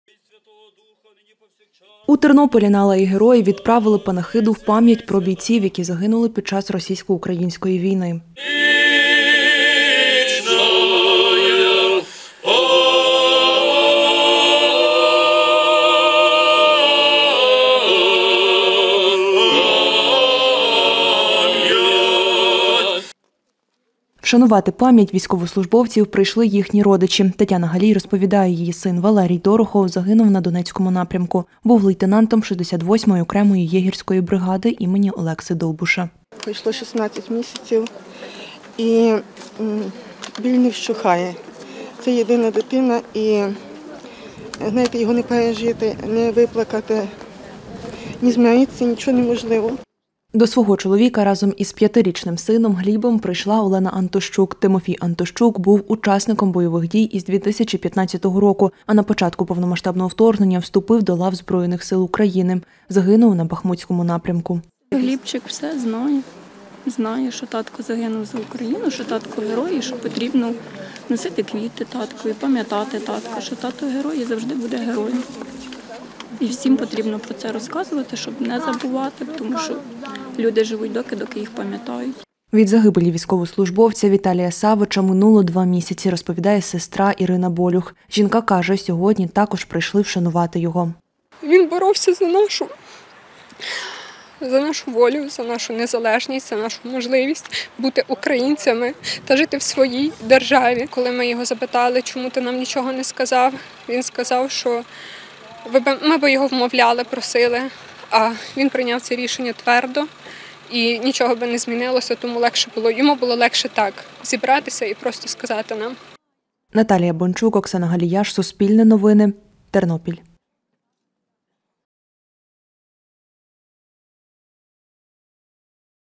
У Тернополі провели панахиду за героями України
У Тернополі до Дня міста помолились за загиблих бійців. На Алеї Героїв зібрались близькі та рідні захисників.